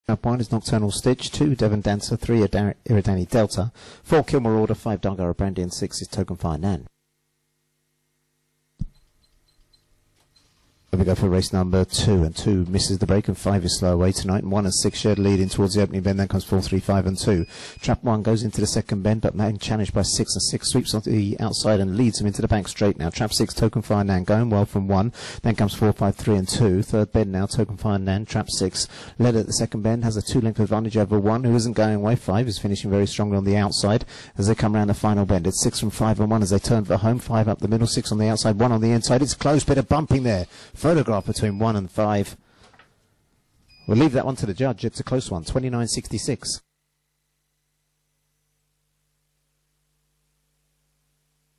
IGB - Tote Trio Pool 26/01/2020 Race 2 - Tralee
Race: Tote Trio Pool
Track Name: Tralee
Date: 26/01/2020
Hour: 18:57

Results

1st    1     NOCTURNAL STITCH    9/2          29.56
2nd    5     DUNGARA BRANDY      6/4 f        29.57
3rd    6     TOKENFIRE NAN       10/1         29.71
4th    4     KILMORE ORDER       11/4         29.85
5th    3     ERIDANI DELTA       9/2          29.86
6th    2     DEVON DANCER        9/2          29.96

|TRL_2020_01_26_02